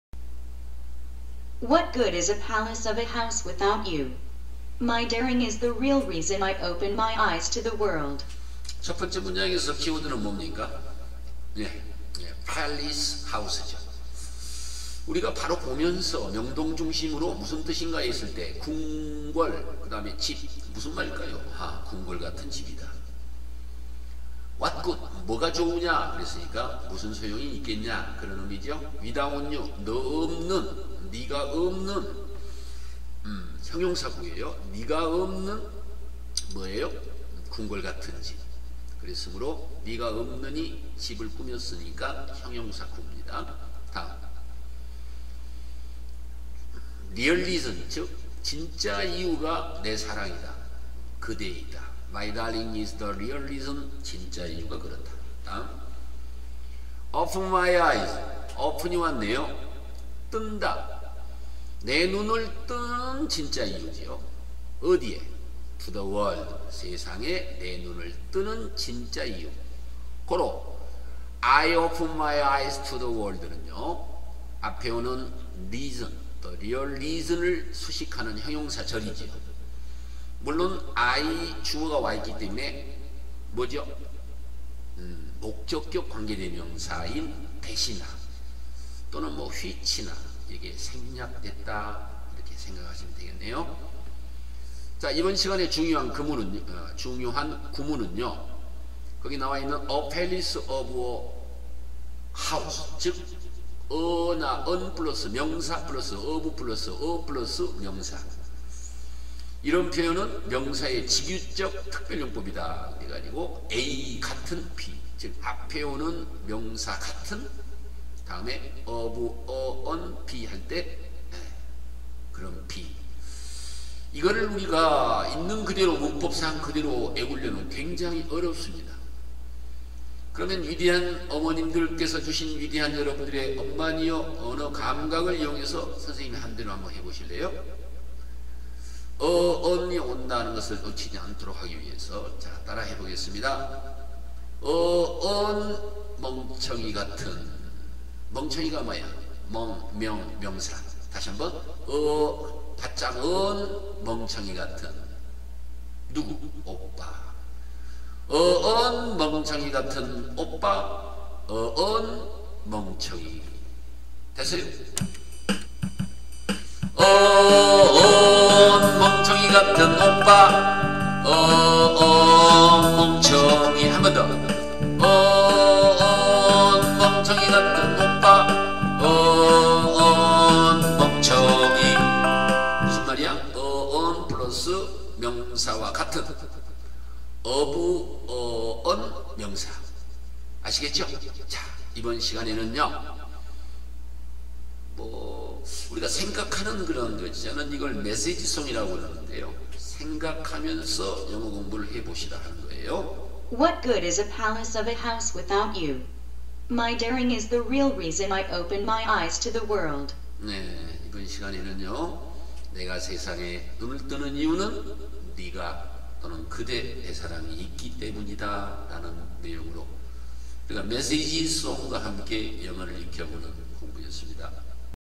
My darling is the real reason
● 명사의 직유적 특별용법
→ 「a / an ⊕ A ⊕ of a / an ⊕ B」꼴은
   「A같은 B」의 의미로 「a / an ⊕ 형용사 ⊕ B」(~같은 B)와 같다.
   물론 A나 B는 명사이며
   처음의 a / an대신에 the, this, that, some, any all, your, my, his와 같은
   「관사/소유격/지시사」 등이 와서 명사의 직유적 표현으로 쓰임으로
   특히 고유명사 앞에 a/an이 오는 것에 주목하기 바란다.
▷ a palace of a house
   (궁궐 같은 집 = a palatial house)
▷ an angel of a wife
   (천사와 같은 아내 = an angelic wife)
▷ a monster of a dog
   (괴물과 같은 개 = a monstrous dog)
▷ a beast of a Mr. Lee
   (짐승 같은 미스터 리 = a beastly Mr. Lee)
▷ a oyster of a man
   (조개와 같이 과묵한 사람 = a oysterlike man)
▷ What good is a palace of a house without you?
   그대 없는 궁궐 같은 집인들 무슨 소용이 있겠어요?

39년 간의 영어교육 현장에서 얻은 Contents로
직접 강의 & 촬영 & 녹음 & 편집 & 교육하고 있는 것들을
YouTube에 공개하고 있습니다.